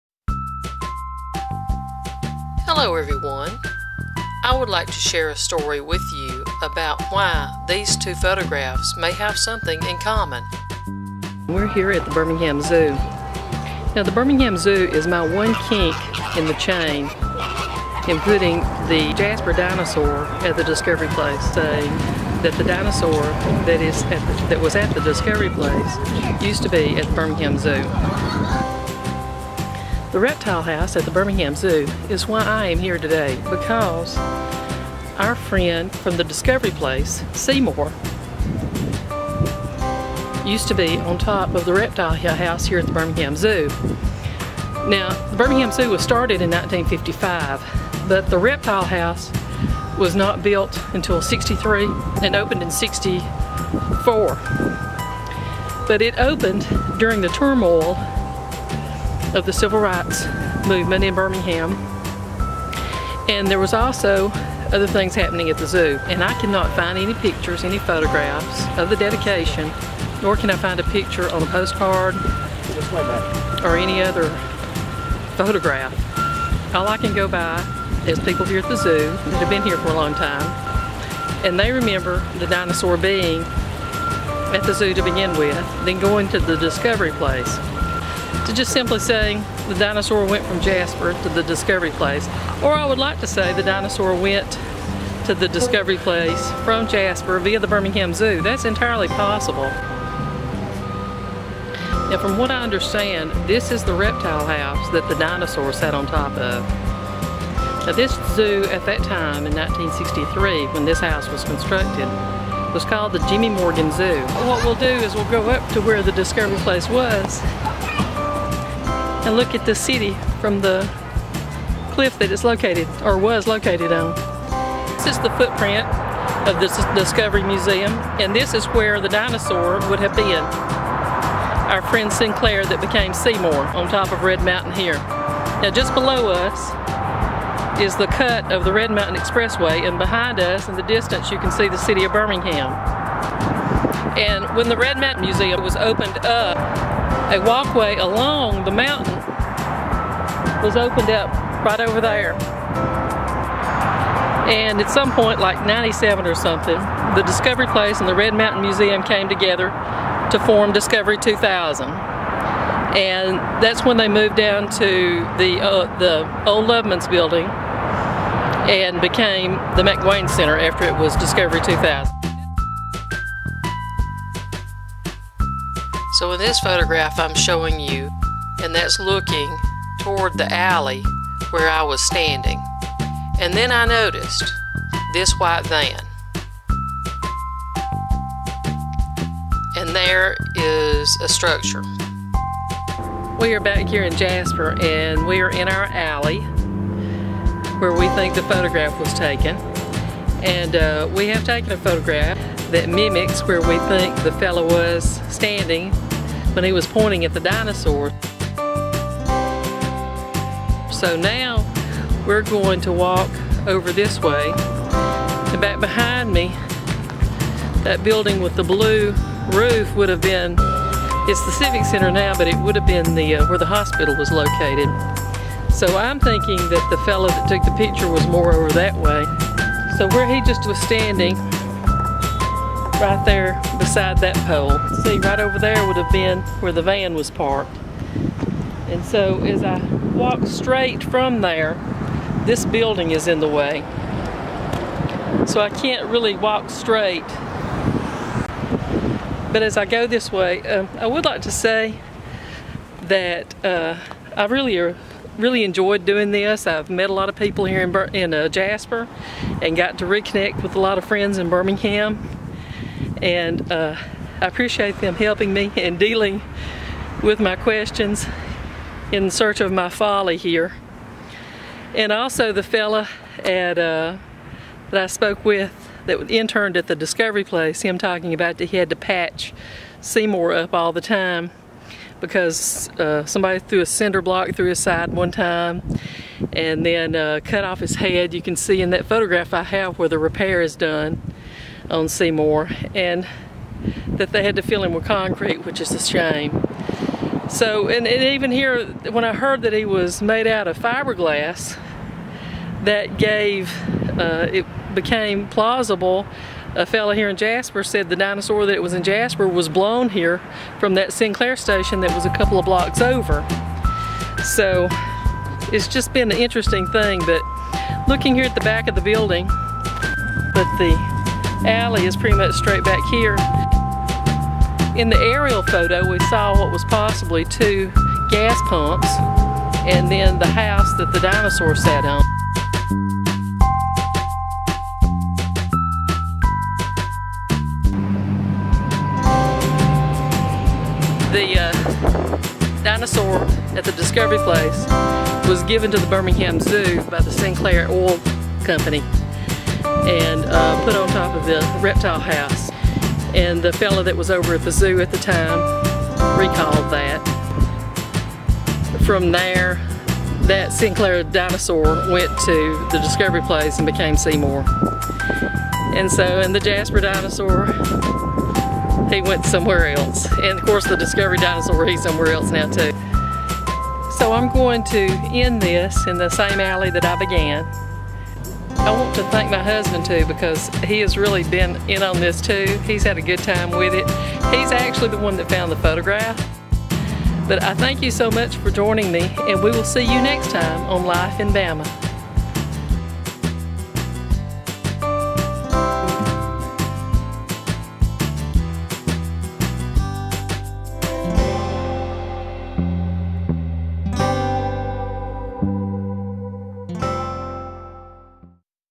The Tornado that hit Jasper with Life In Bama
The Dinosaur Mystery Quest Solved is Part Two of "Did the Dinosaur Do It?" To get up to speed on this watch the video I made on Jan. 13th
We begin at the Birmingham Zoo to see the location of Sinclair that was at there.  We follow him to the Discovery Place and learn more about that place. But was the Dinosaur in Jasper this Sinclair? Did the Dinosaur that was in the Jasper Tornado destruction end up at the Birmingham Zoo? We reconstruct the scene of the photograph we found at the Carl Elliott Regional Library to see where the Dinosaur was located in Jasper.